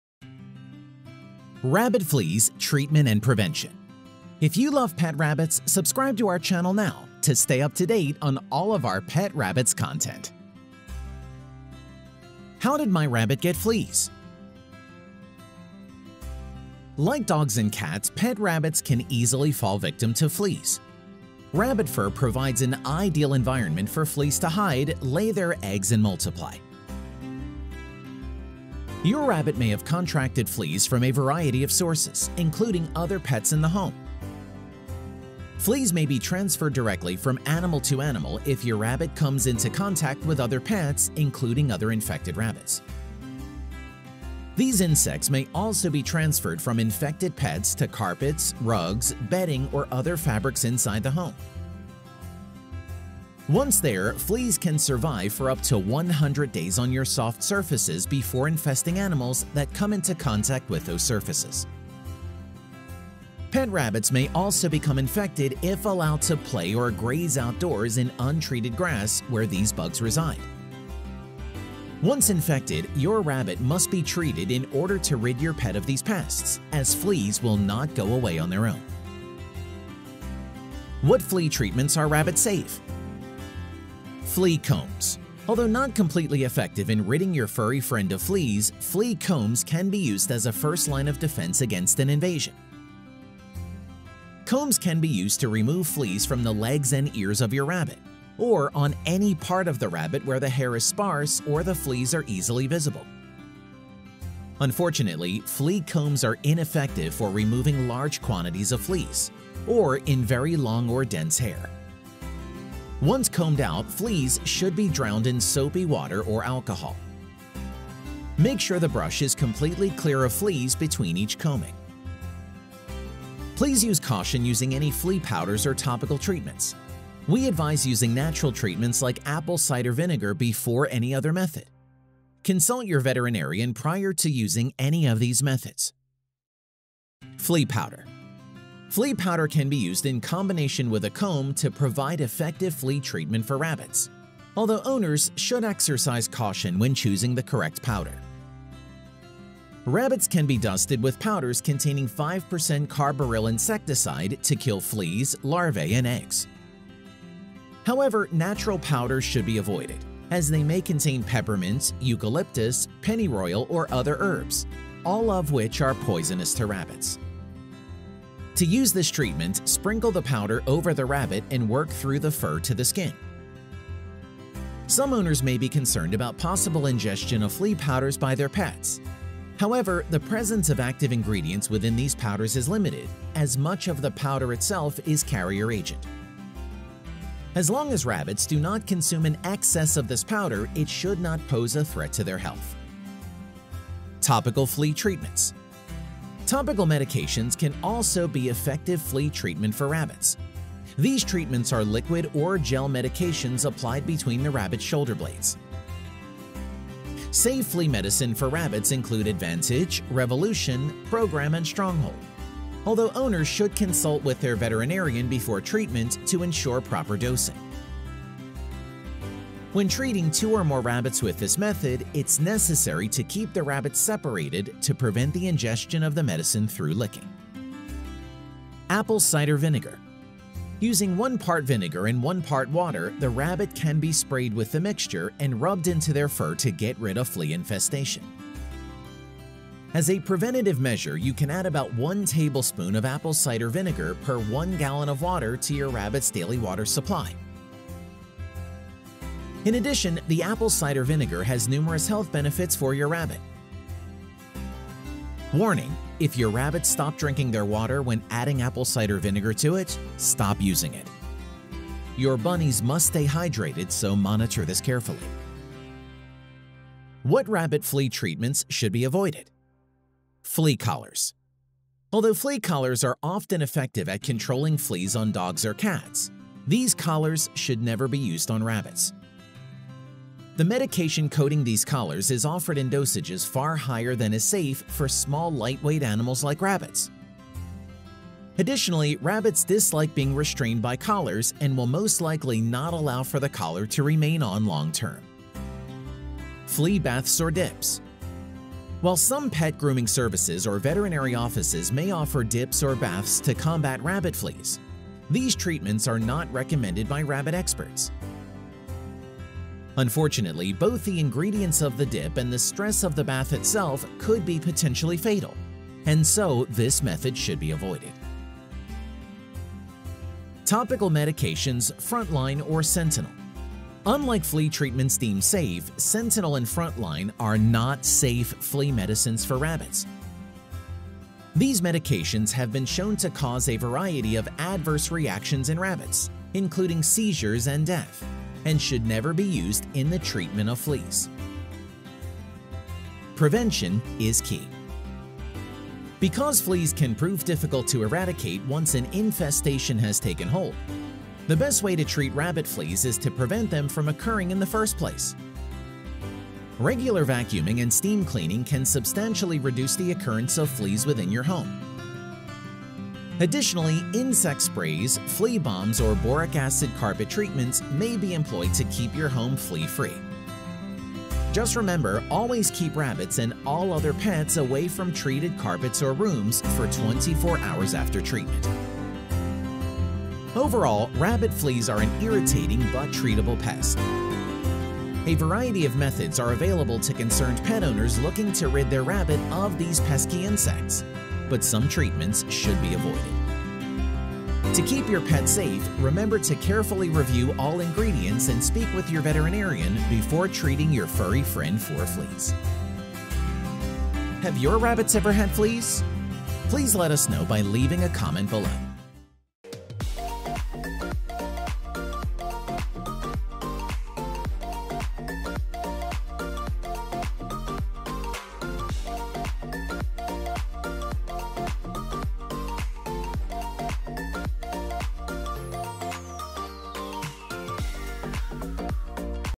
Rabbit Fleas: How To Get Rid of Rabbit Fleas and Prevent Them From Ever Happening Again!
How did my rabbit get fleas?

Like dogs and cats, pet rabbits can easily fall victim to fleas. Rabbit fur provides an ideal environment for fleas to hide, lay their eggs, and multiply.

Your rabbit may have contracted fleas from a variety of sources, including other pets in the home. Fleas may be transferred directly from animal to animal if your rabbit comes into contact with other pets, including other infected rabbits. These insects may also be transferred from infected pets to carpets, rugs, bedding, or other fabrics inside the home. Once there, fleas can survive for up to 100 days on your soft surfaces before infesting animals that come into contact with those surfaces. Pet rabbits may also become infected if allowed to play or graze outdoors in untreated grass where these bugs reside. Once infected, your rabbit must be treated in order to rid your pet of these pests, as fleas will not go away on their own.